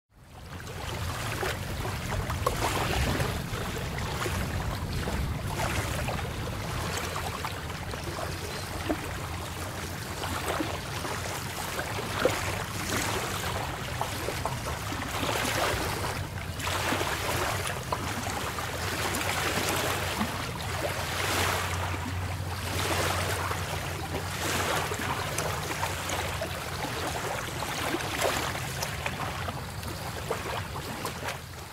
A cute Labrador fetching stick
The Labrador Retriever (simply Labrador, or Lab for short) is one of several kinds of retrievers, a type of gun dog. Even-tempered and well-behaved around young children and the elderly. Labradors are athletic, playful, and the most popular breed of dog.